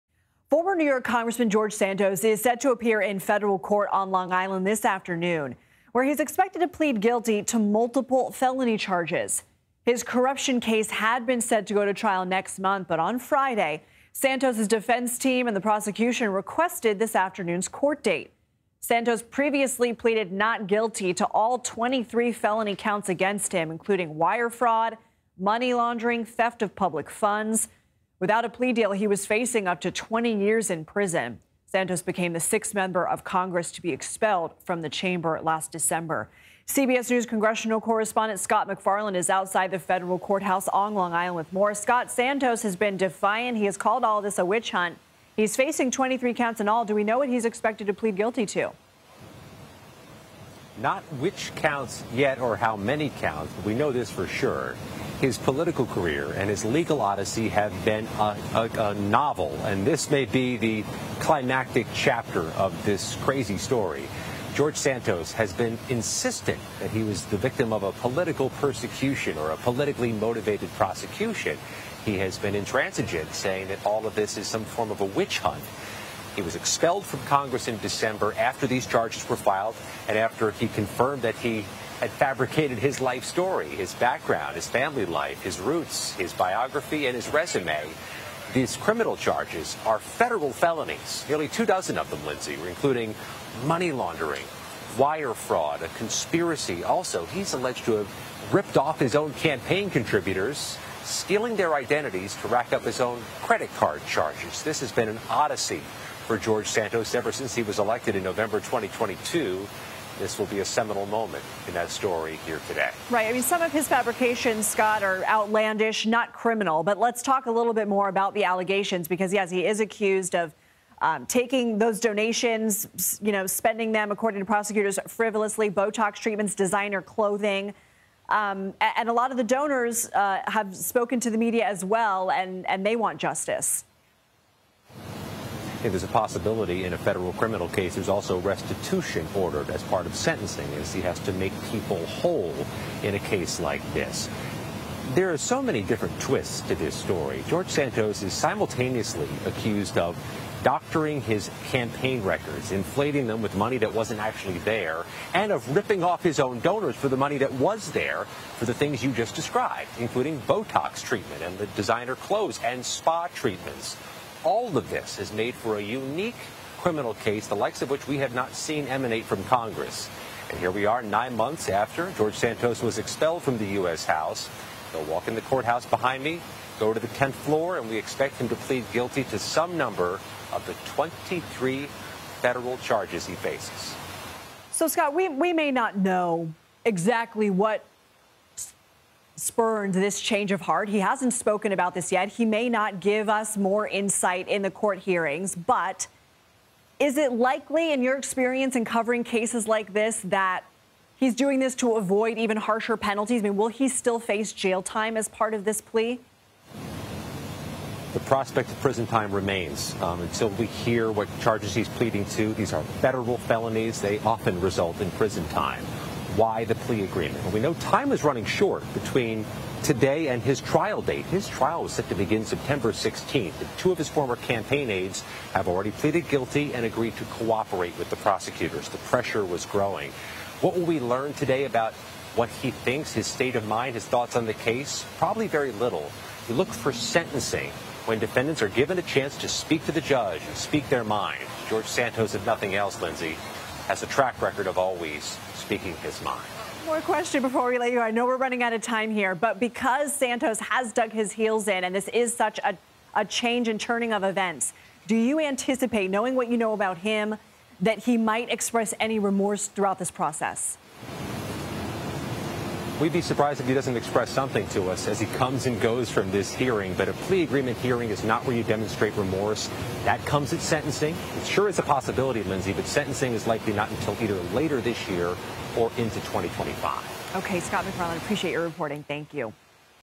George Santos guilty plea expected
Former New York Rep. George Santos is expected to plead guilty to several felony fraud charges Monday, CBS News has learned. Santos has been facing criticism for fabricating details about his past and allegedly misusing donor credit card information. CBS News' Scott MacFarlane has more.